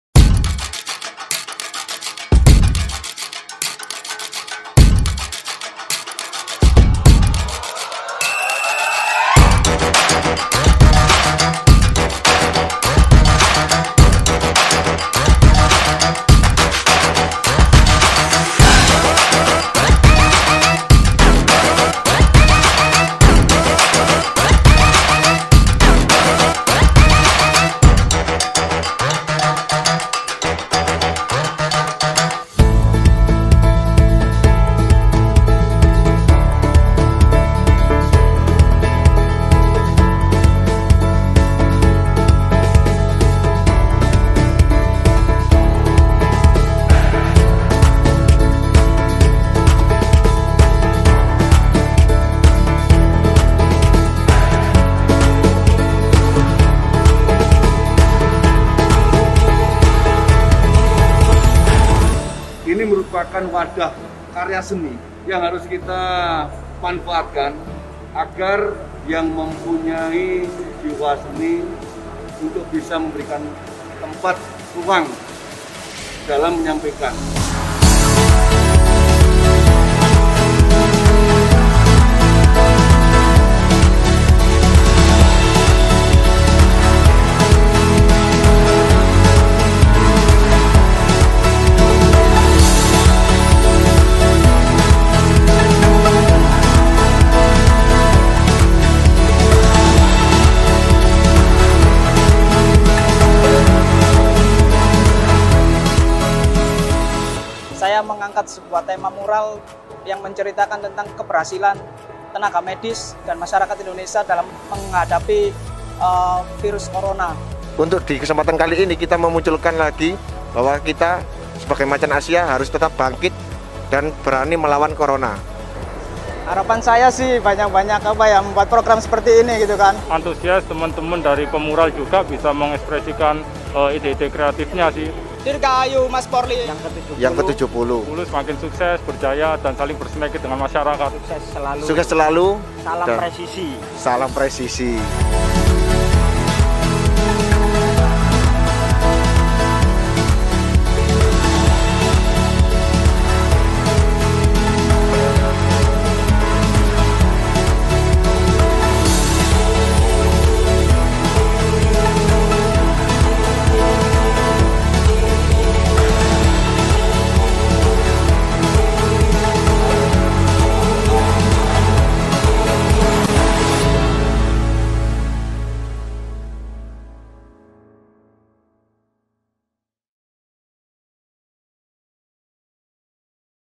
BHAYANGKARA MURAL FESTIVAL 2021 POLDA JATIM
HARI JADI HUMAS POLRI KE-70

Rangkaian Kegiatan Bhayangkara Mural Festival dalam rangka HUT ke-70 Humas Polri di Mapolda Jatim